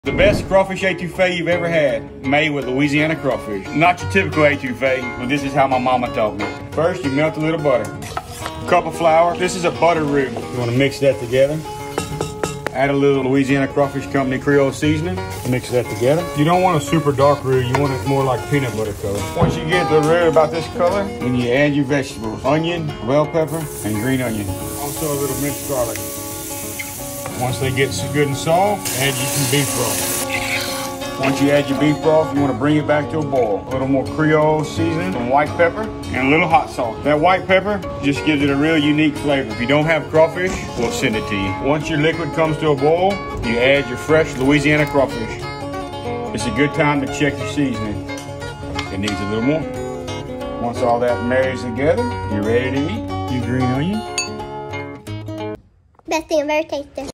Louisiana Crawfish Étouffée
Ingredients & Method Below👇🦞

Ingredients:
* 32oz Beef broth
* 1 1⁄2 Stick(s) butter
* 1 Pound(s) crawfish tails w/ fat
* 3/4 cup of flour
* 3 Pod(s) garlic, chopped fine
* Creole seasoning to taste
* 1 onion chopped
* 1 green bell pepper chopped
* hot sauce to taste
* white pepper to taste
* green onion whites
Method:
* Melt butter in a skillet.
* Add flour and creole seasoning to make a roux. Stir until blended and the color of peanut butter.
* Add onion, bell pepper, green onion whites, and garlic. Cook until tender.
* Add beef broth and bring to a boil. Then reduce heat, add crawfish tails, and cover.
* Cook for 15-20 minutes on low heat.
* Add white pepper and creole seasoning to taste.
* Simmer until the seasoning blends.
* Serve over rice and garnish with green onions or parsley.